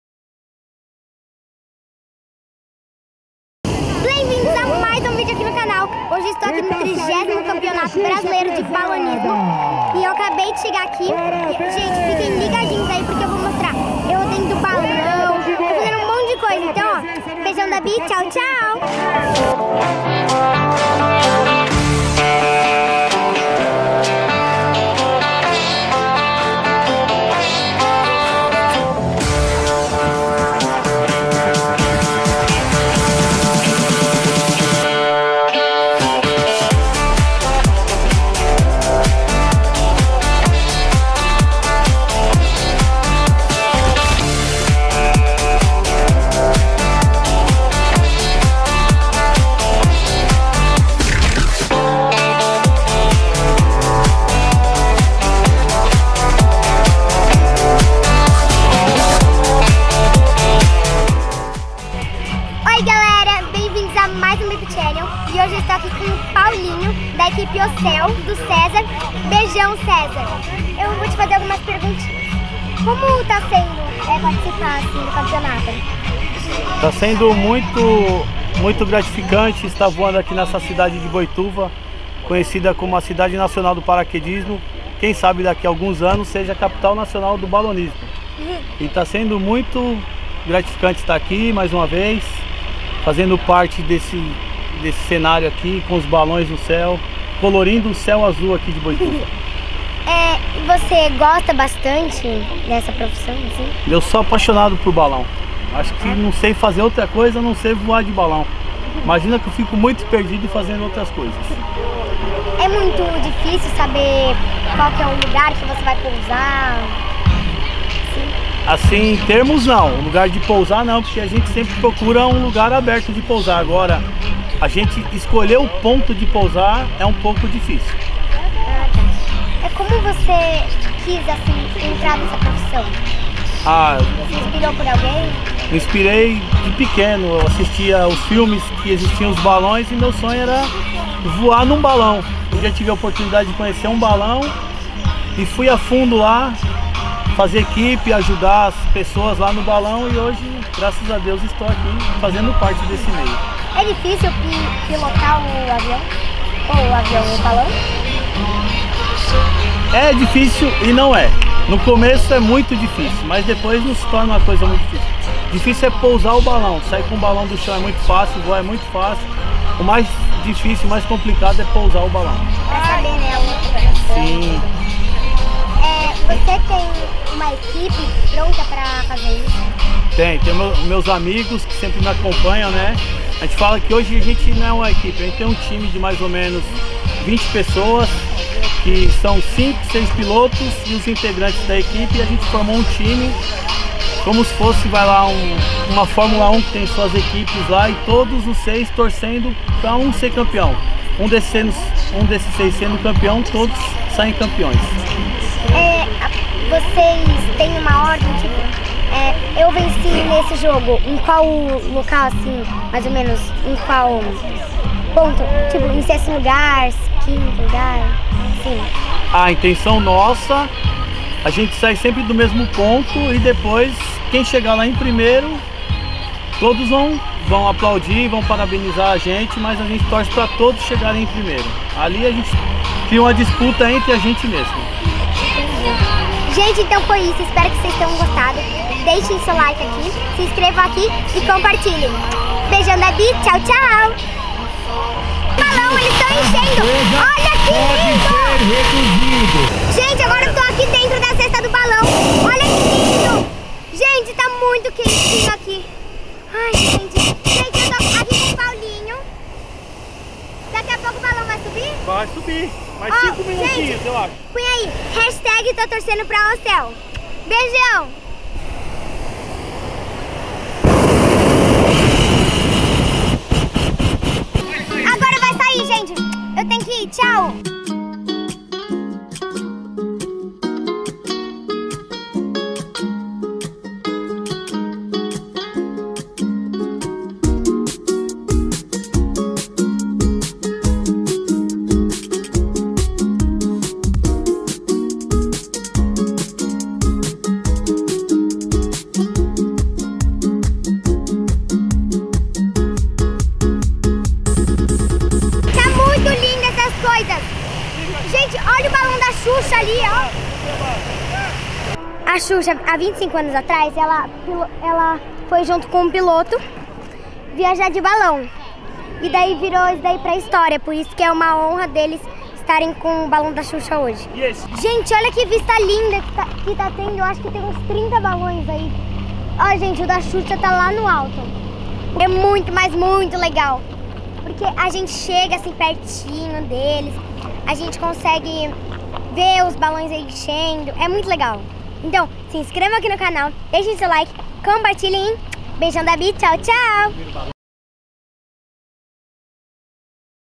Voei de BALÃO? 🎈 30o. Campeonato Brasileiro de Balonismo - Bibbi Channel - Bia Brumatti
Bia Brumatti participa do 30- Campeonato Brasileiro de Balonismo aonde entrevista o campeão Marcos Paulo e mostra com exclusividade o Famoso Balão da Xuxa.

Beijão da Bia e Tchau Tchau!!